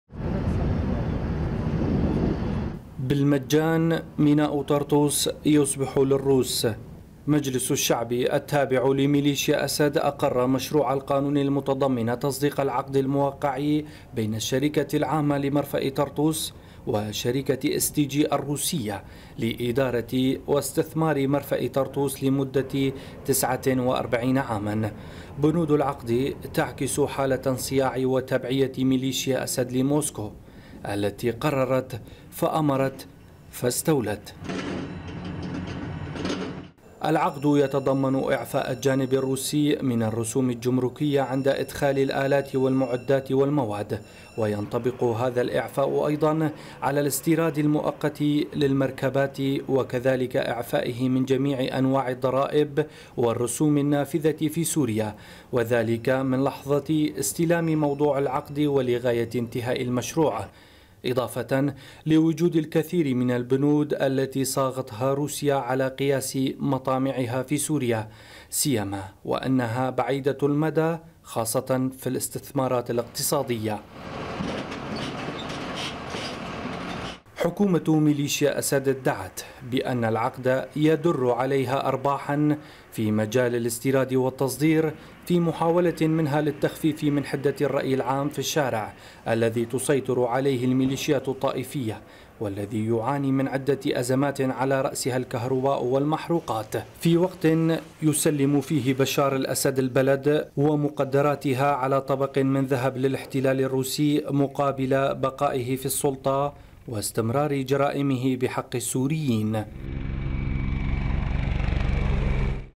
تعرف إلى بنود عقد تسليم ميليشيا أسد ميناء طرطوس لروسيا - سوريا
أقر مجلس الشعب التابع لما يسمى بنظام الأسد تسليم ميناء طرطوس للاحتلال الروسي لمدة تسعة وأربعين عاما، وذلك ضمن عقد أظهر مدى التبعية لموسكو والانصياعِ لأوامرها، يأتي ذلك في وقت يعاني فيه موالو النظام من عدة أزمات على رأسها الكهرباء والمحروقات.
تقرير: أحمد الحسن